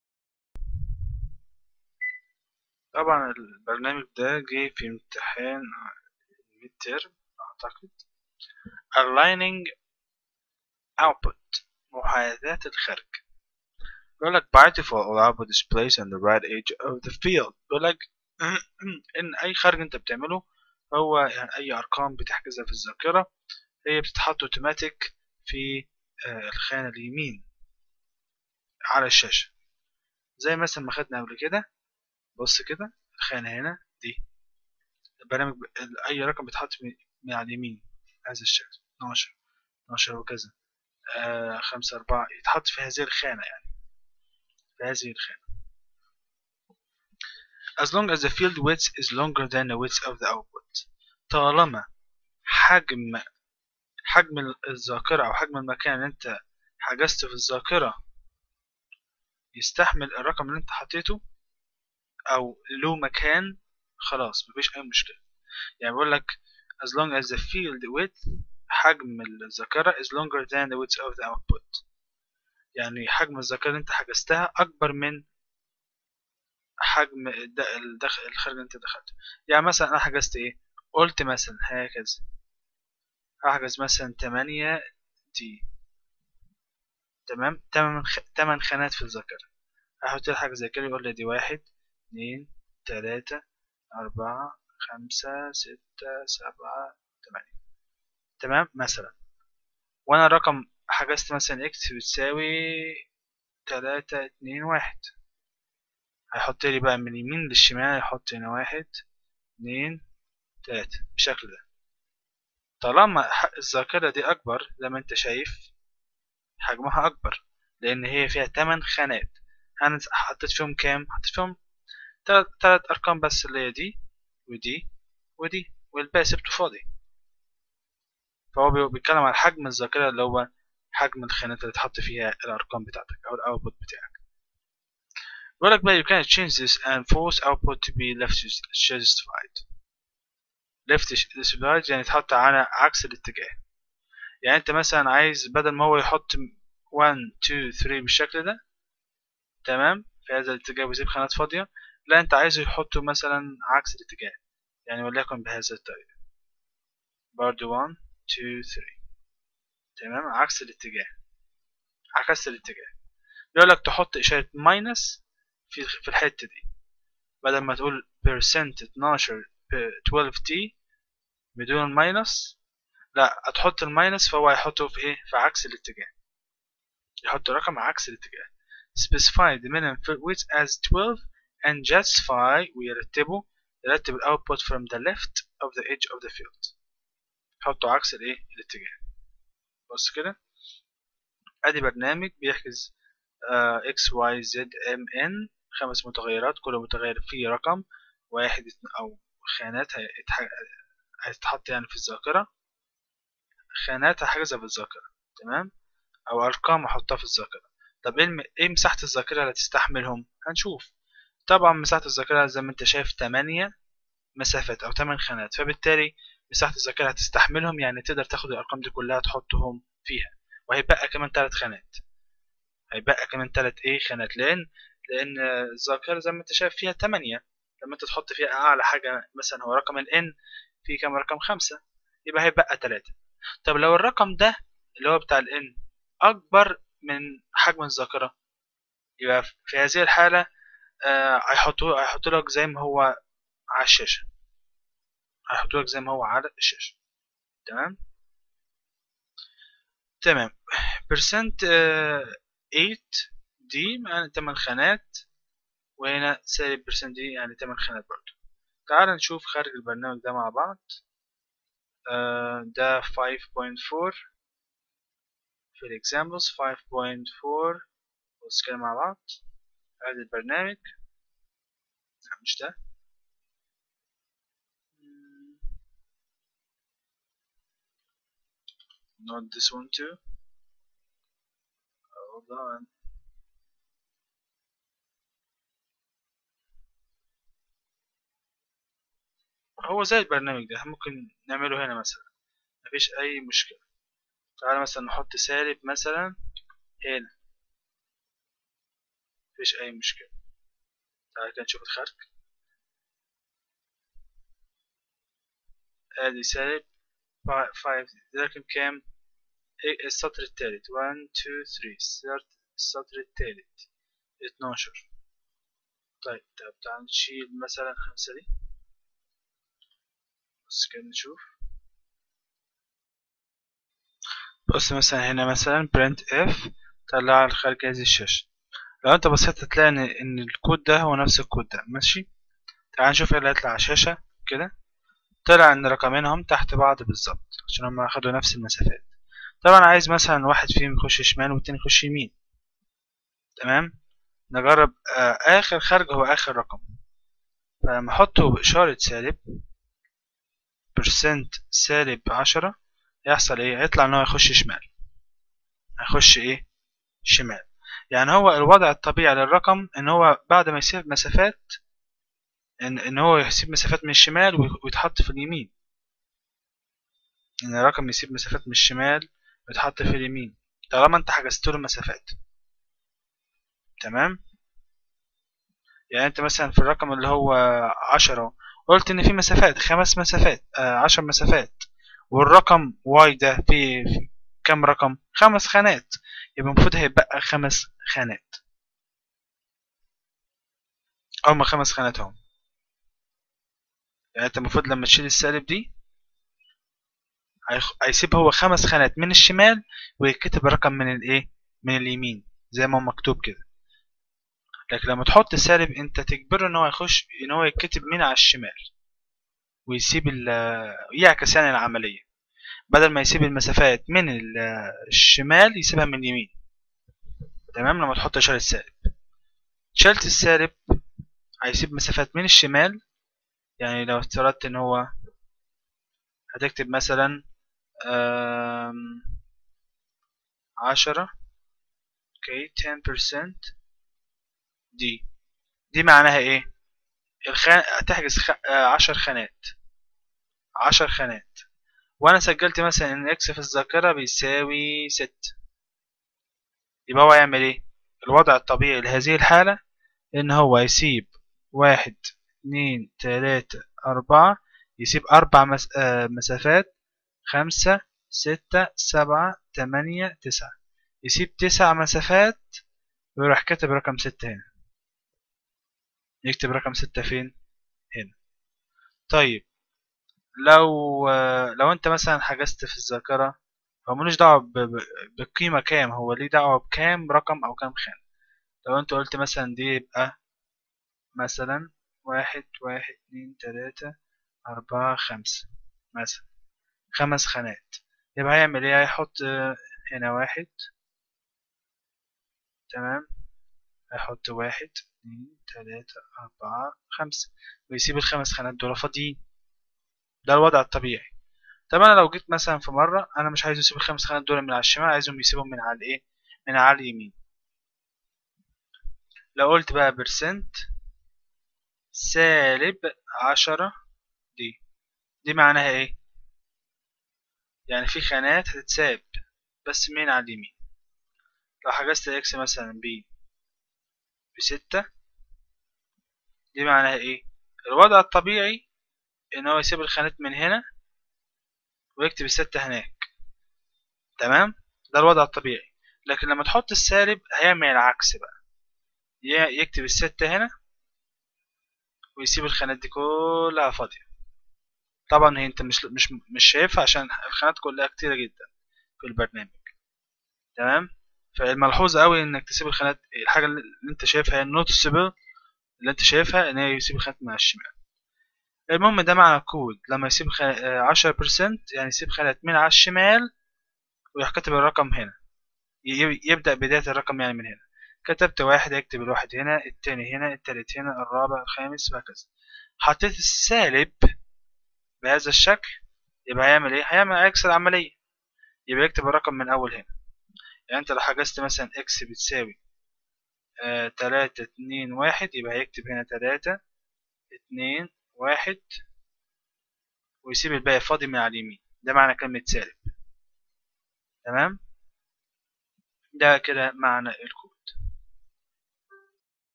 Alignment output كيفية عمل محاذاة للخرج بلغة السي %d or %5d or %-5d
لتحميل منهج لغة C الذي اقوم بشرحه كامل بالبرامج من الملف ده